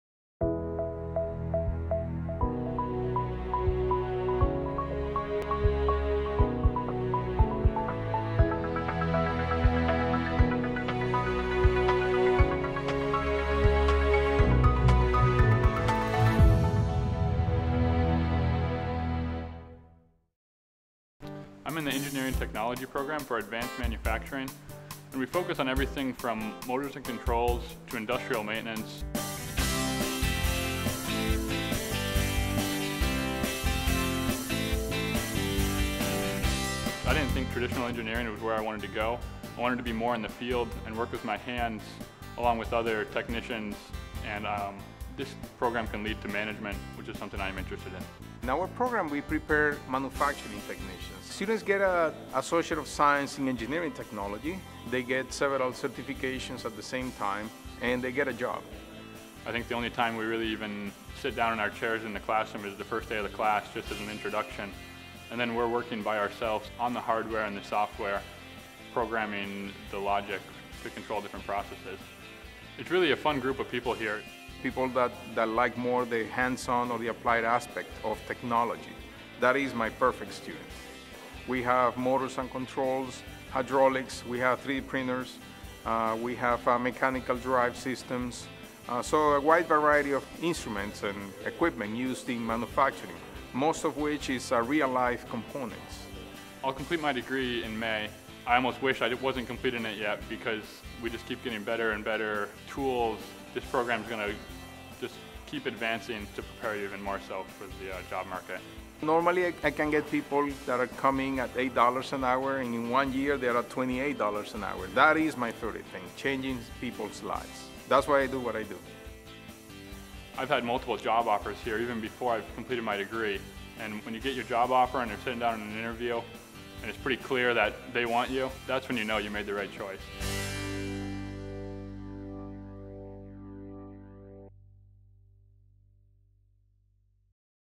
Engineering Technology
Engineering Technology is a comprehensive program covering introductory computer-aided drafting, electronics, instrumentation and testing, processes and materials, quality and safety. These skills align with the national Manufacturing Skill Standards Council (MSSC) Portable Production Technician certification. The engineering technology curriculum, which emphasizes advanced manufacturing, prepares students for many high skill/high wage/high demand jobs in manufacturing and other high-technology industries.